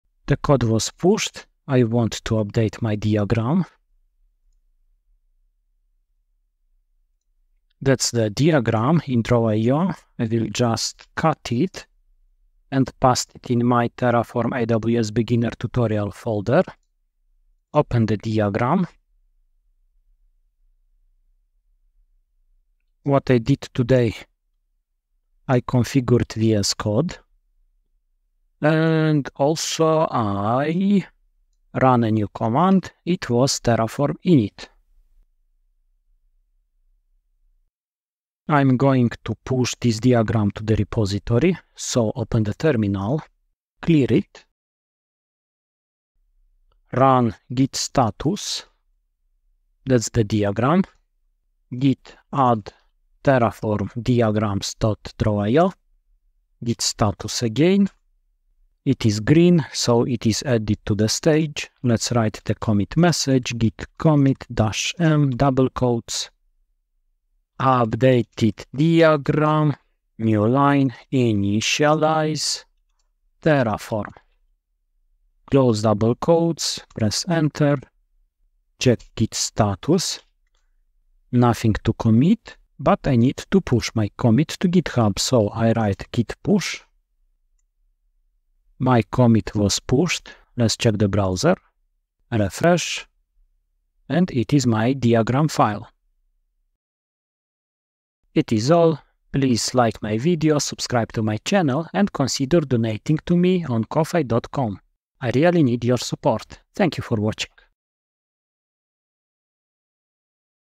Terraform+AWS, part 3.6 | Initialize terraform | Update progress diagram | Beginner’s tutorial
Chapters:
00:00 Update diagram
00:38 Push to GitHub
01:37 Ending

Special thanks to:
 • Adobe
 • Audacity
 • Canva
 • Kdenlive
 • OBS